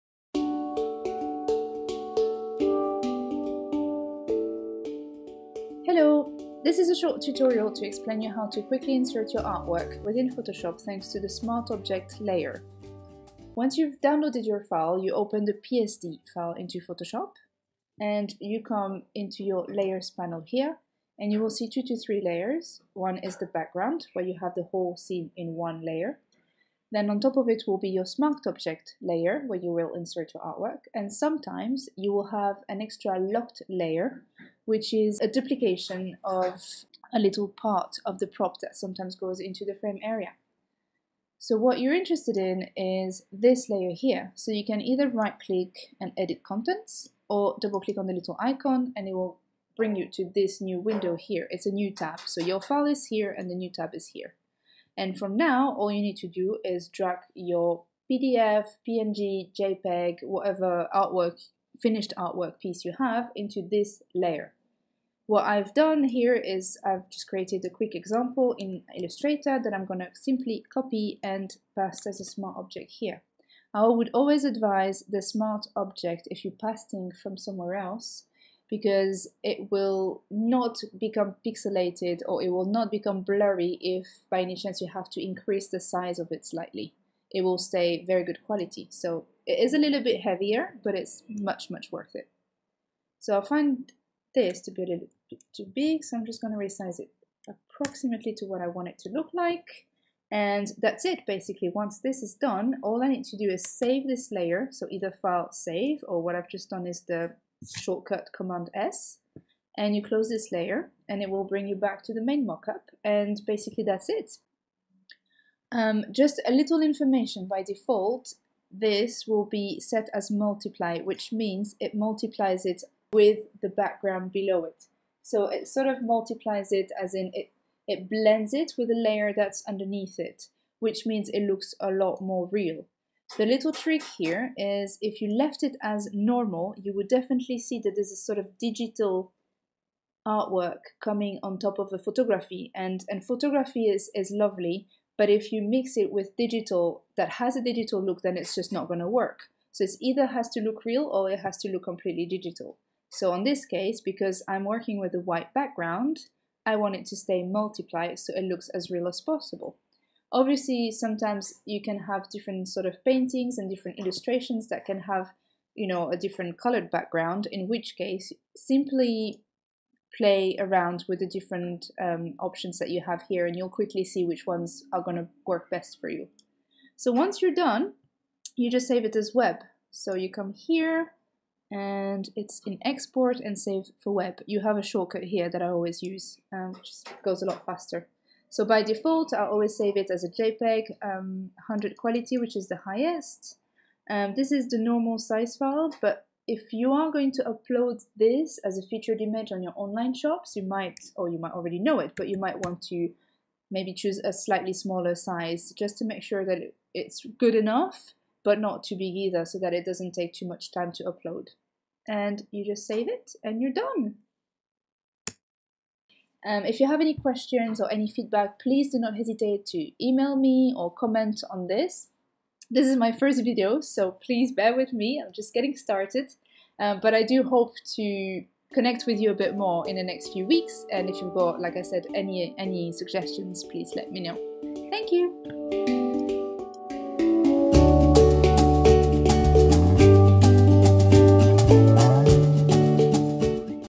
Insert your art in a mock up using Photoshop Smart Object layers
You just bought a mock up but not too sure how to achieve the best effect just yet? This is a mini video on how to quickly insert your artwork within Photoshop thanks to the Smart Object layer.

This video is a tutorial requested by clients who purchased White Hart Design Co. mockups or want to learn how to use Smart Object layers that come within mockups in Photoshop.

Ideal for creative businesses, artists, illustrators, calligraphers, letterers, photographers and bloggers.

UPDATE 2021: I am so grateful to see this helped so many of you! I am preparing other videos to help you out with mockups and online shop launches and development.